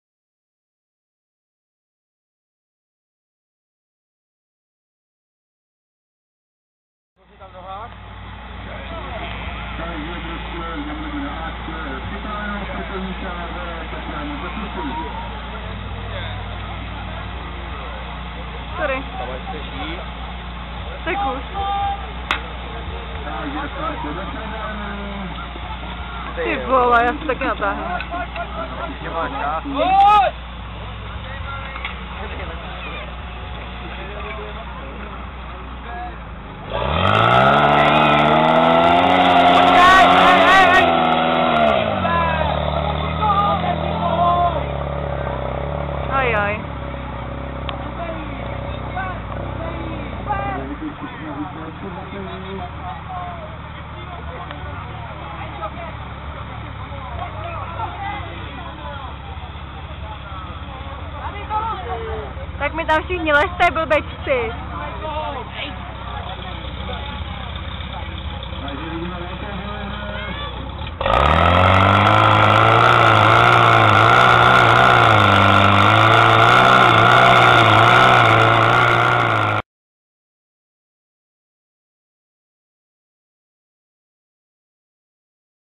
SDH Dobronín: Požární útok Ženy A - Okrsková soutěž, Věžnička 2011
SDH Dobronín: Požární útok Ženy A - Okrsková soutěž, Věžnička (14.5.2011)
100 let SDH Věžnička
výsledný čas: 59,73s